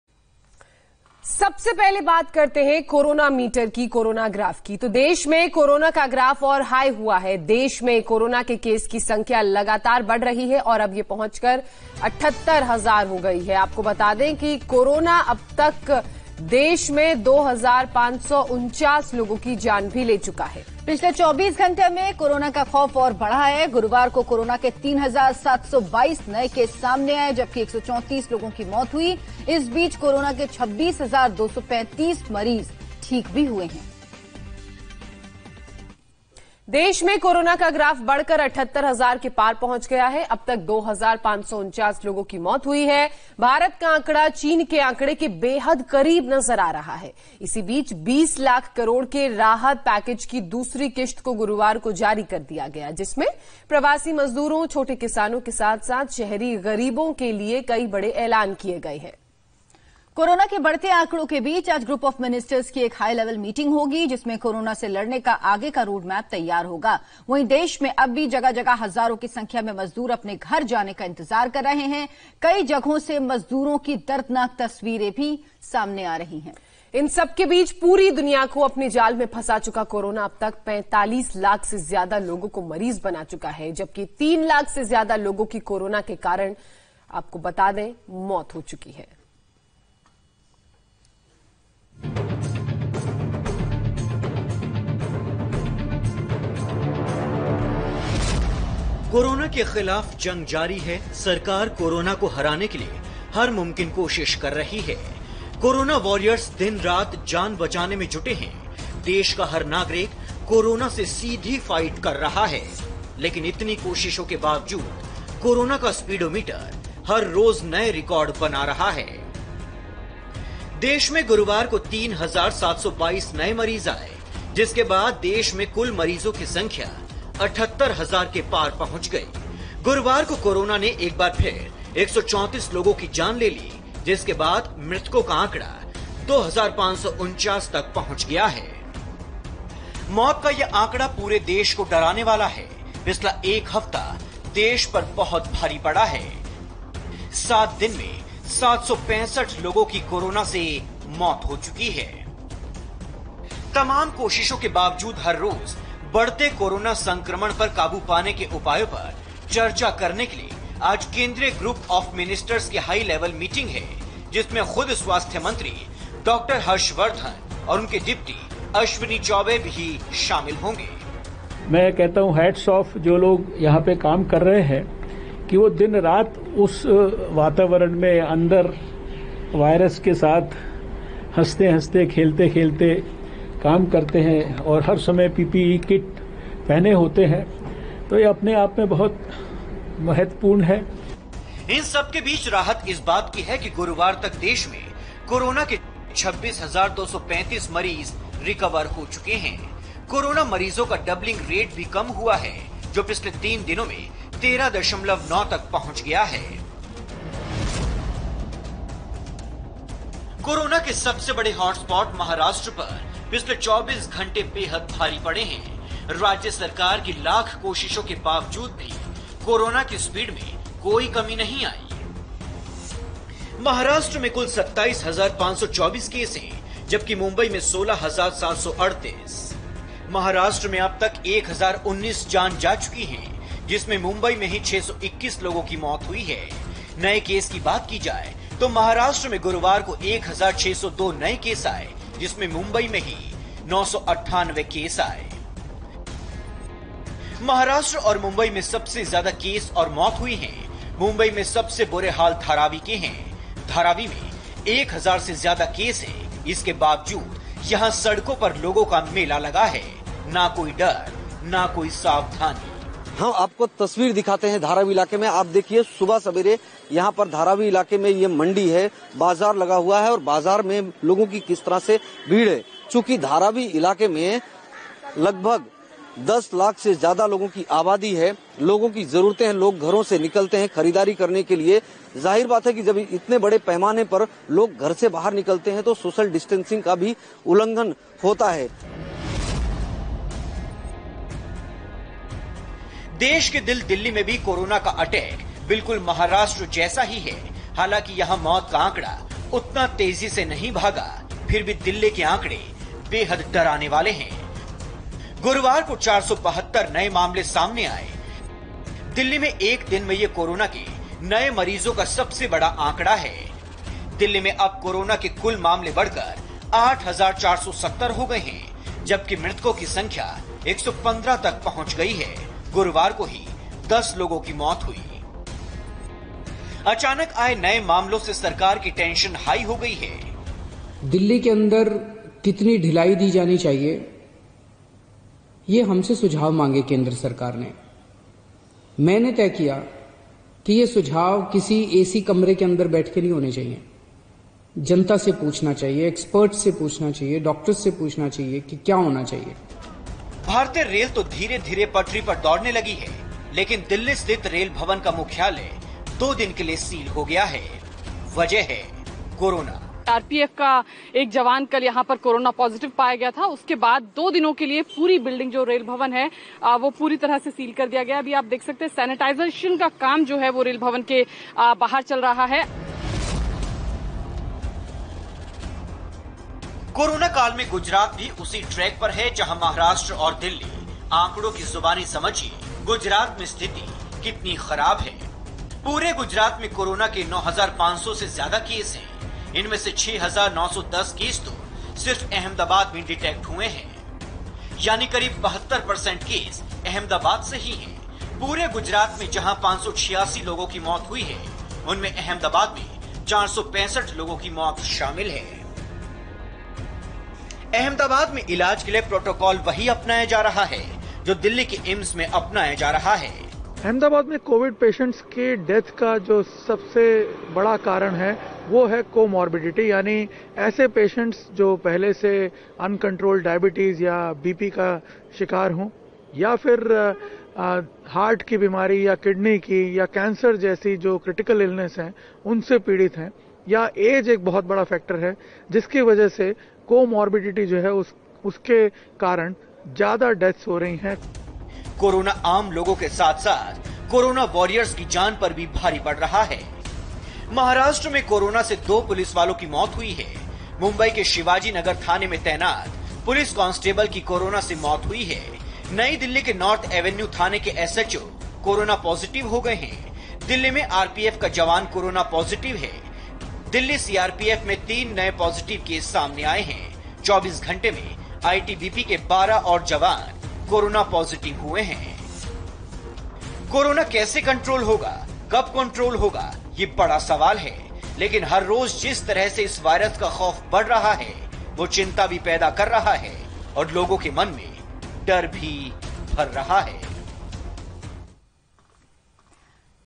Coronavirus In India Updates: Cases Top 78,000; Over 2,500 Dead | IndiaTV News
Total number of confirmed coronavirus cases in India crossed 78,000 with over 2,500 dead, as the Prime Minister’s CARES fund poured in more money to combat the virus. India added 3,722 new Covid-19 cases over 24 hours, according to the Union Health Ministry’s 8:00 a.m. update on May 14. The total number of cases in India stands at 78,003. This includes 26,235 people who’ve recovered and 2,549 deaths.